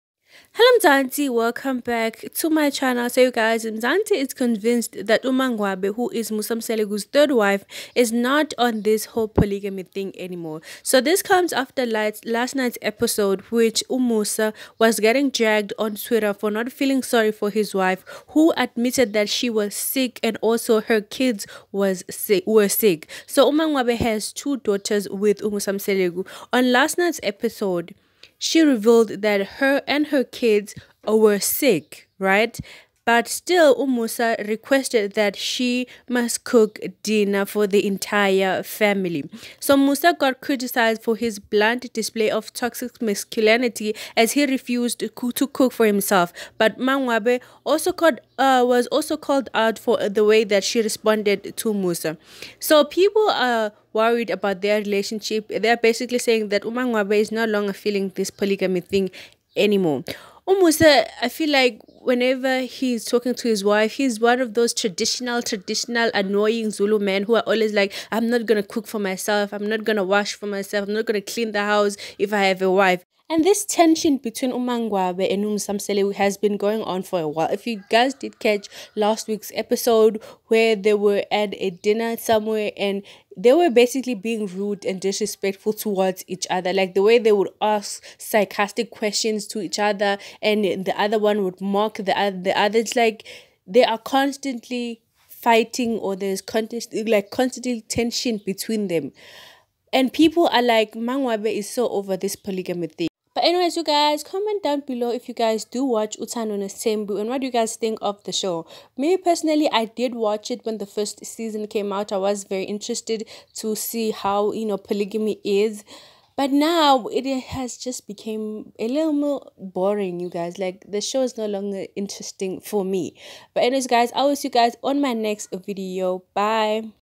Is Mangwabe's Marriage With Musa Mseleku Over??
Mzansi is convinced that MaNgwabe, Musa Mseleku's third wife, is not into this whole polygamy thing anymore. That is after last night's episode which saw Musa getting dragged for not feeling sorry for his wife, who admitted that she and the kids were sick.